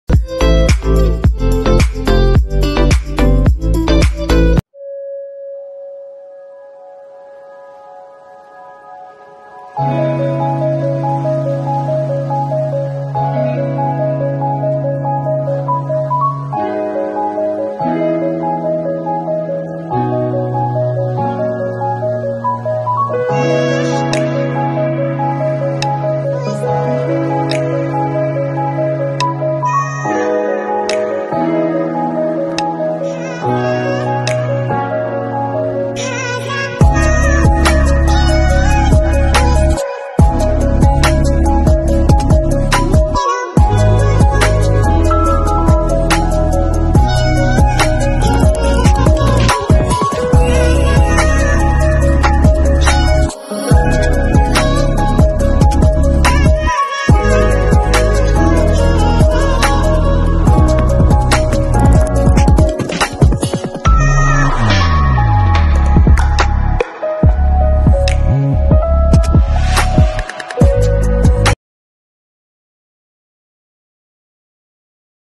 Lee's Ice Cream Factory
Today, we will be showing you guys Lee's Ice Cream Shop!